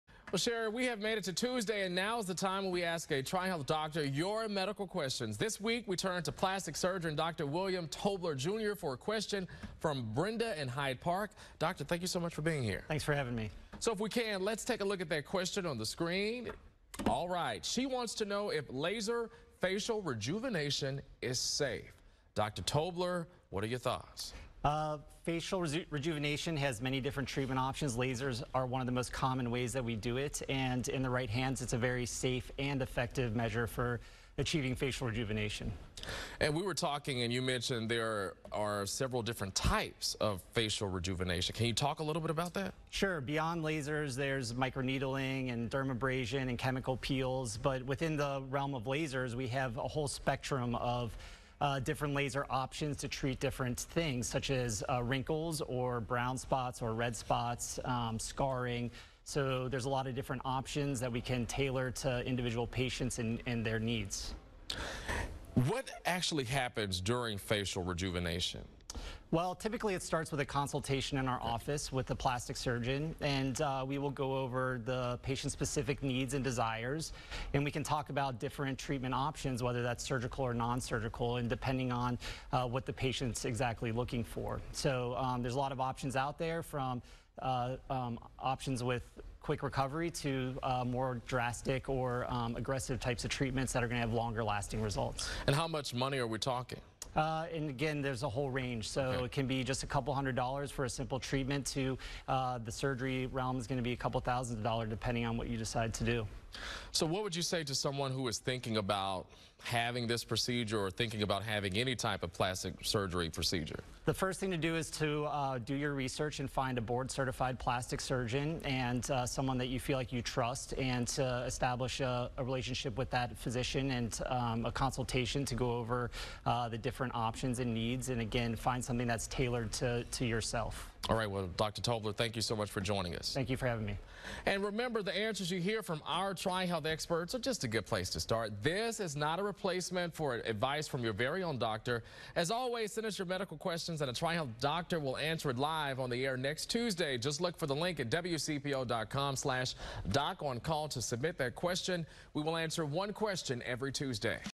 Doctor on Call: Is Laser Facial Rejuvenation Safe?
There are many different facial rejuvenation treatment options, with laser treatment being one of them. But is it safe? That is the question submitted for this segment of TriHealth’s Doctor on Call by Brenda in Hyde Park. William Tobler Jr., MD, a plastic surgeon with the TriHealth Cosmetic Surgery & Rejuvenation Center answers her question and shares not only a few other options for facial rejuvenation, but explains what specific conditions can be treated, which options are best for each situation and what happens during the procedure. He also addresses concerns about cost and what steps you should take if you are considering facial rejuvenation.